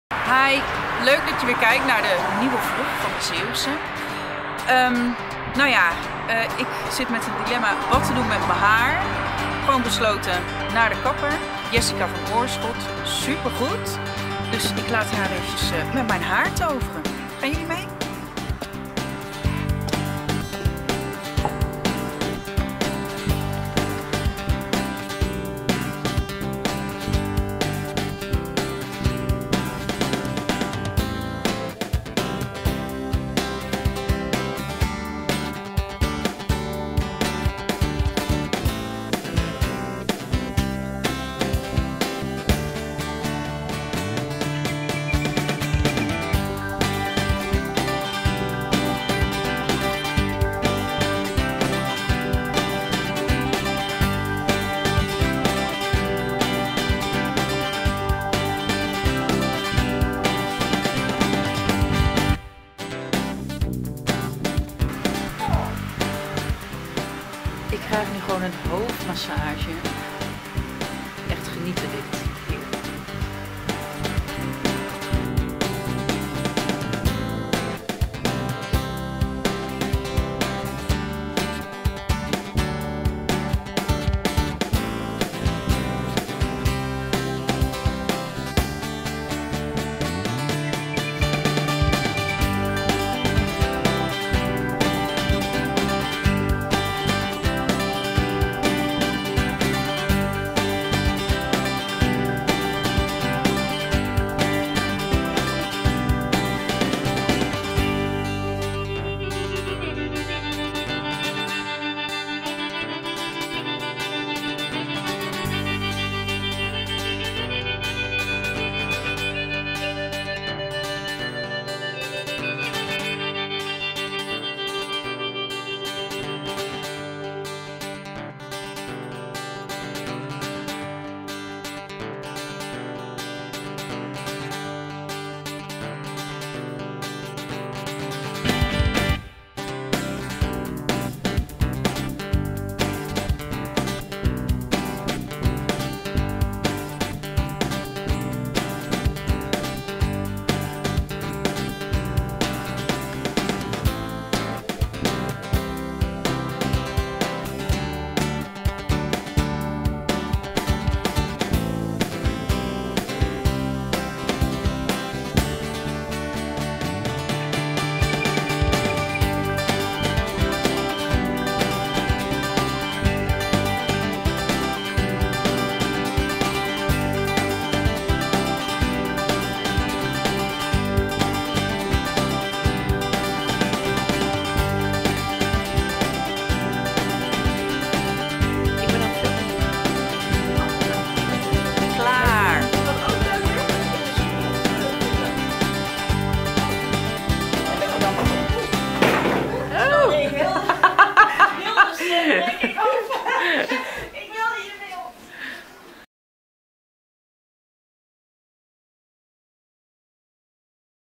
Naar de KAPPER met Gaby!!!! Lekker kort koppie???
Who is a girls best friend?
Gaby neemt jou mee naar de kapper en kiest voor een kortere coup😄

Eigenzinnige Zeeuwse meisjes die b(v)loggen over alles wat met Zeeland te maken heeft, MAAR soms nemen we de boot naar het vaste land!!